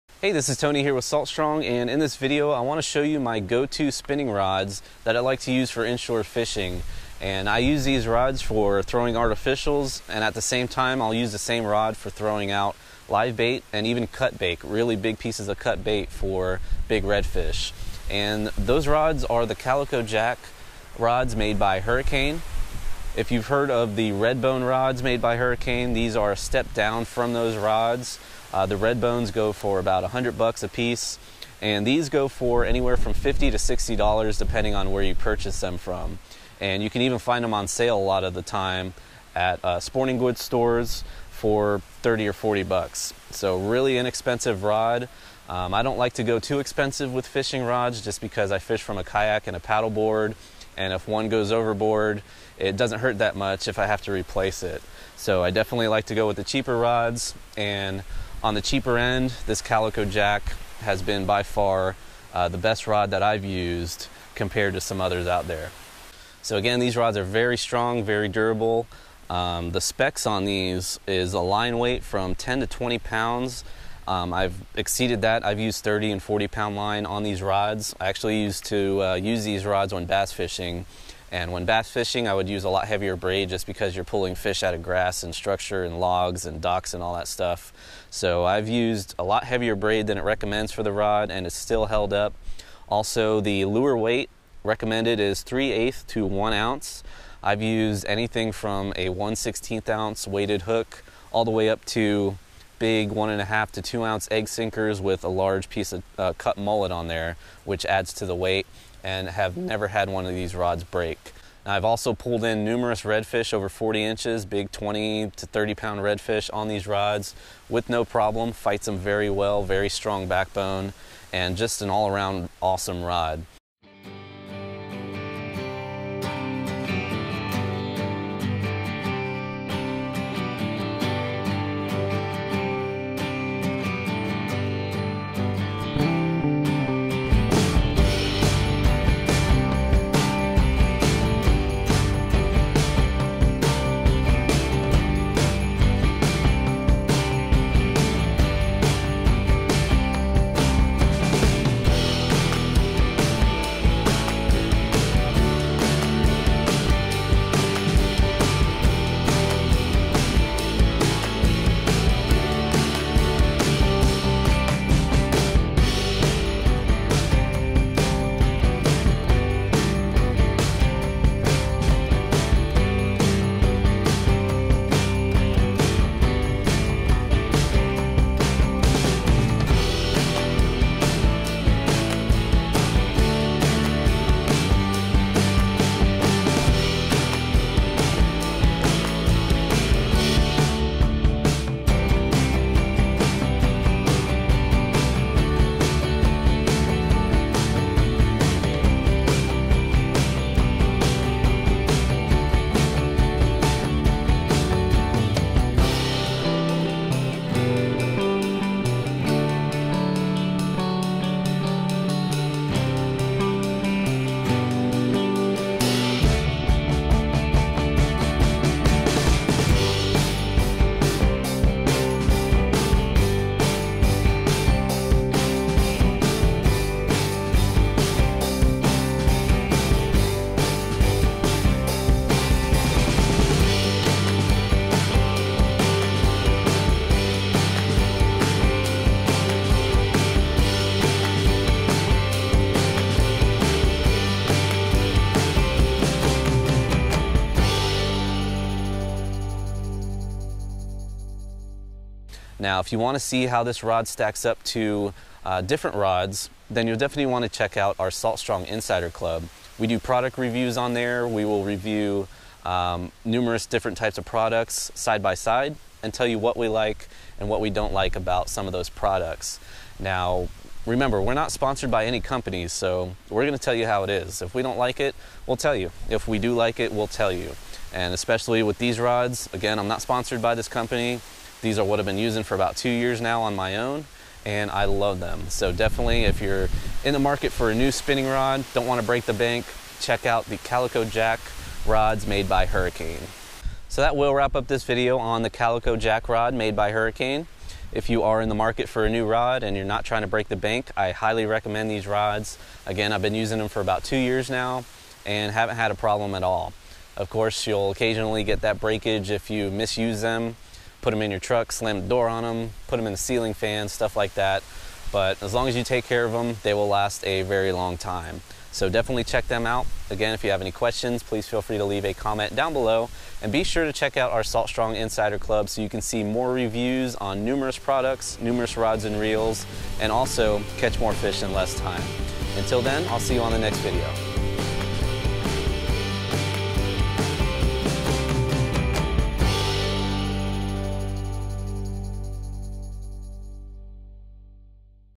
Have You Tried This Inshore Rod? (Calico Jack Review)
Here at Salt Strong, we are always on the lookout for the best inshore fishing rods (in particular, inshore spinning rods).

One that we've really put to the test is the Calico Jack spinning rod made by Hurricane (same company that makes the Redbone fishing rods).

It's certainly priced right (you can find these rods in the $50-$60 range), and it's proven to be incredibly tough and versatile (which is what I am looking for when I am fishing from my kayak or paddleboard since I can only carry a rod or two that I need for many purposes.

So far, this Hurricane Calico Jack inshore spinning rod has helped me land monster bull redfish, tarpon, and pretty much every type of inshore fish found in Florida.

And not only is it built well, it also does a great job of casting everything from light soft plastics all the way to heavy cut bait (which is why it's so versatile).

I don't think this is the only inshore spinning rod you ever want to own, but for kayak and paddle board anglers who fish a lot and go through a lot of rods, this is one of the best deals I seen so far.

What are your favorite inshore fishing rods?

Fish On!

To learn more about the Salt Strong Insider Fishing Club, click here now: